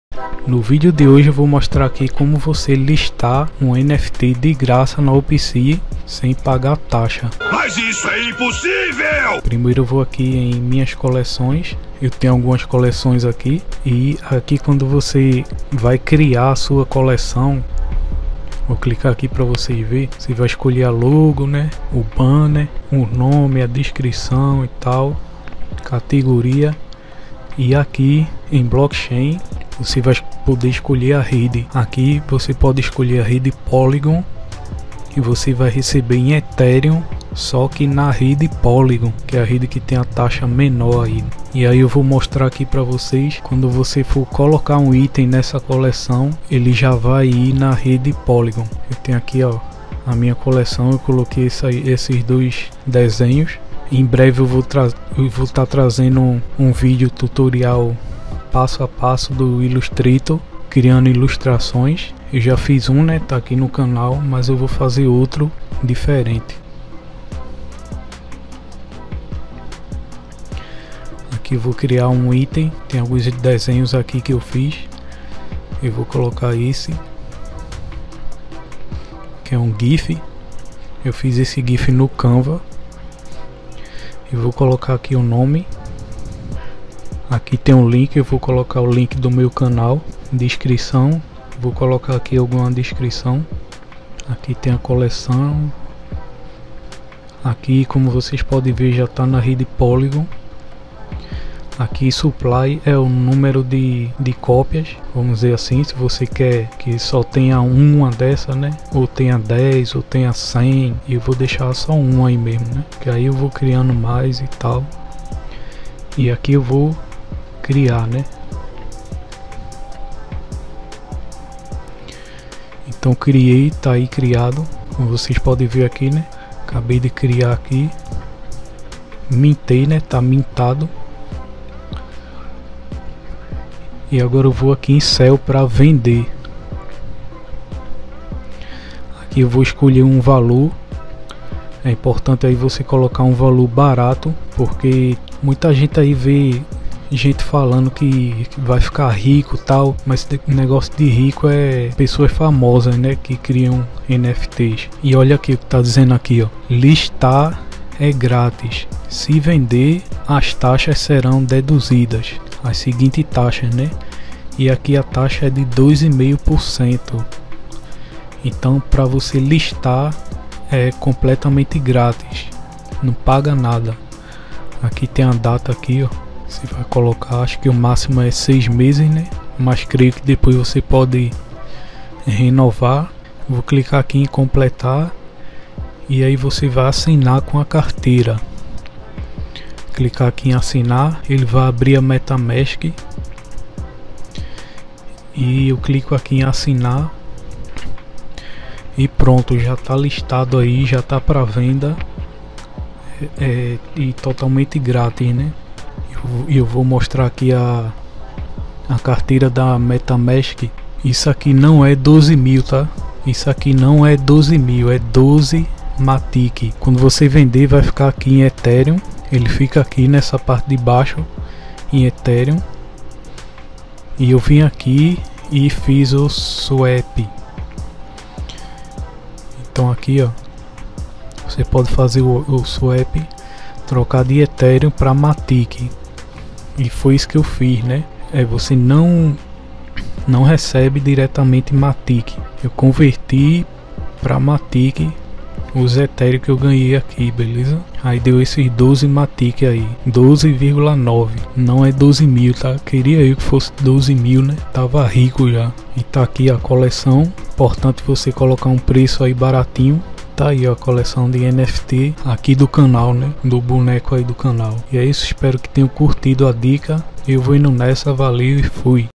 Como Listar um NFT de Graça na Opensea (rede polygon)
Neste vídeo eu mostro como listar uma NFT de graça na opensea usando a rede polygon, na rede Polygon pra listar é grátis, só paga uma taxa pequena se vender.

Sou eu Matheus
Opa e aí, eu sou o Matheus
Neste canal eu trago dicas úteis, meus hobbies e outros assuntos.